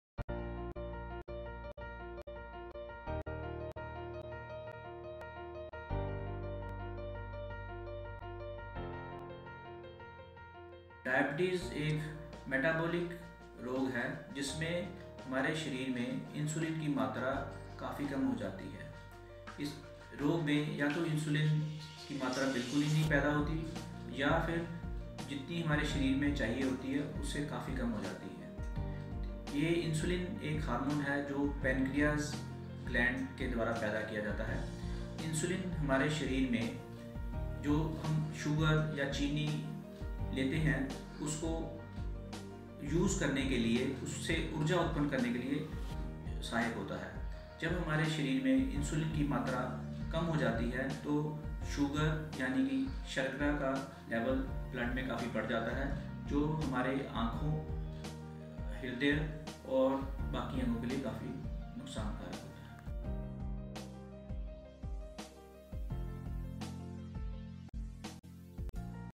Dr. Ashu Arora 2/3
Disha for Diabetes Awareness: Diabetes is one of the most Dangerous and rapidly growing diseases in India. Dr. Ashu Arora, leading Diabetologist explains more in this video. Please watch and share!